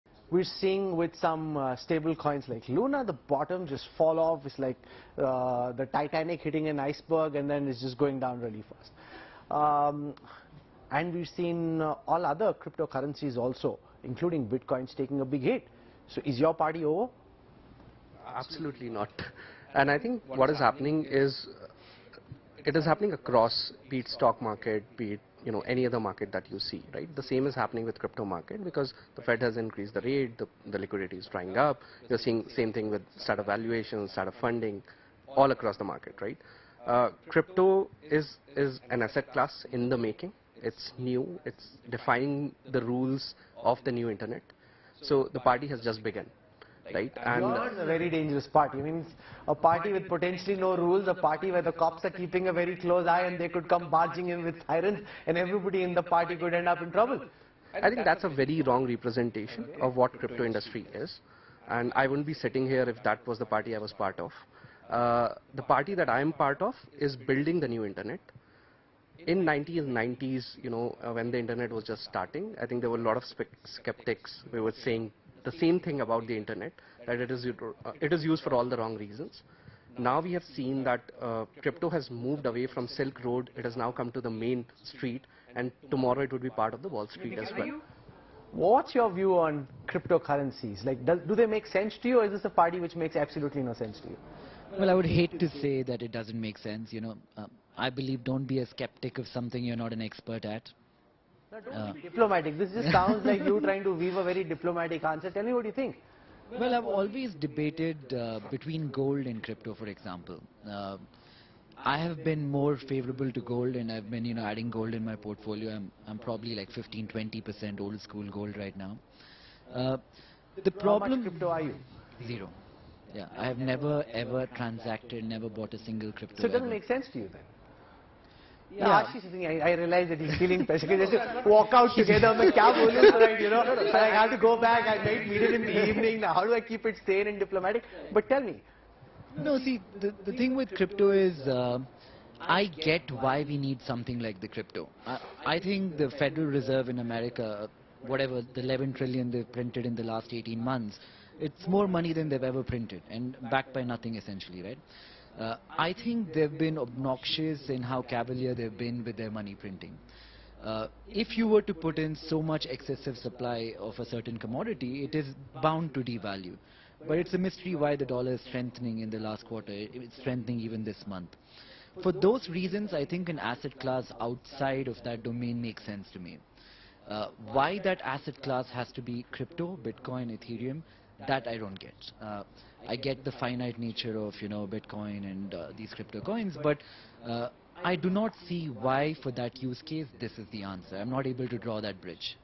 'The Party Has Just Begun': CEO CoinSwitch Kuber Ashish Singhal Talks About Scope Of Cryptocurrency
Davos Brainstorming 2022 with Rahul Kanwal: Speaking to India Today's Rahul Kanwal on start-up businesses, India's top start-up tycoons talk about the scope of cryptocurrency and much more: